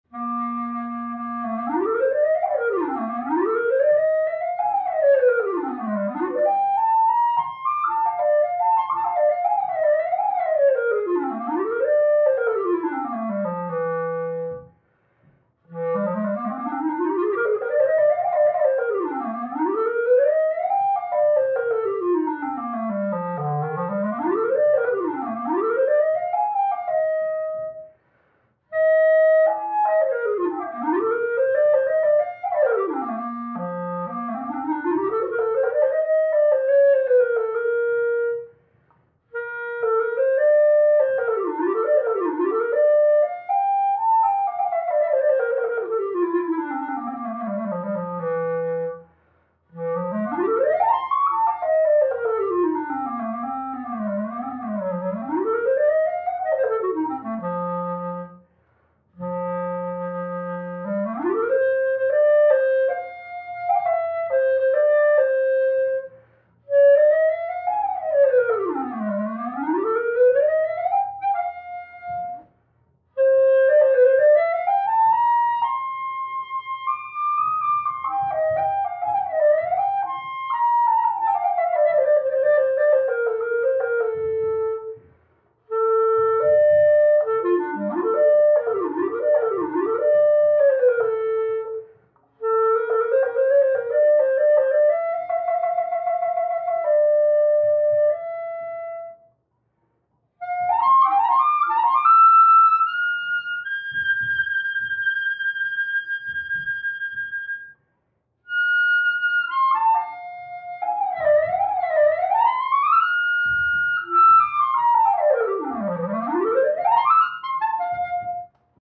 Armando Vazquez Clarinet
Armando Vazquez Clarinet performs in the city of New York on 14th floor